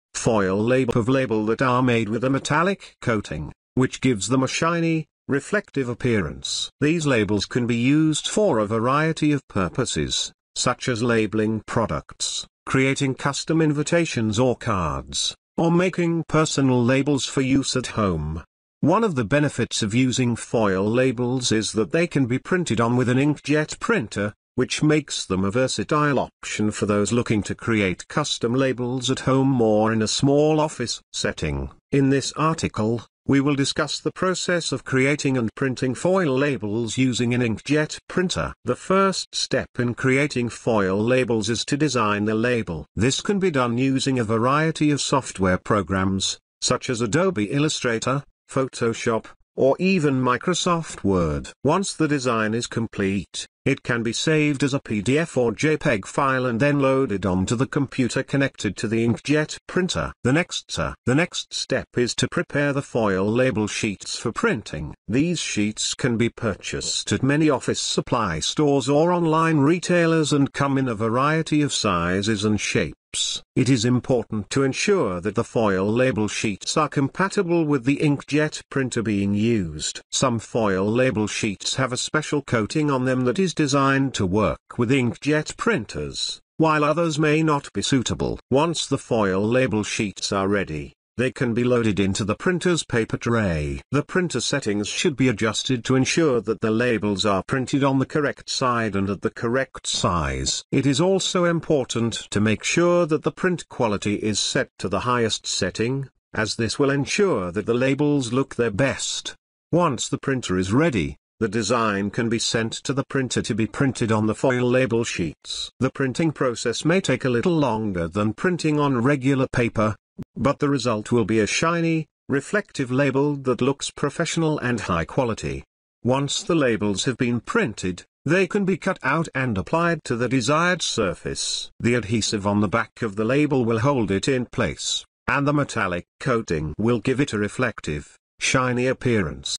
Foil Labels For Inkjet Printer
Foil labels for inkjet printers are special types of labels that have a metallic or foil finish. They are made of a material that is compatible with inkjet printing technology and can be used for a variety of applications, such as product labeling, packaging, and branding. These labels can be printed with high-quality text and graphics and are resistant to fading, smudging, and water damage. They are also available in a range of colors, sizes, and shapes to suit different needs.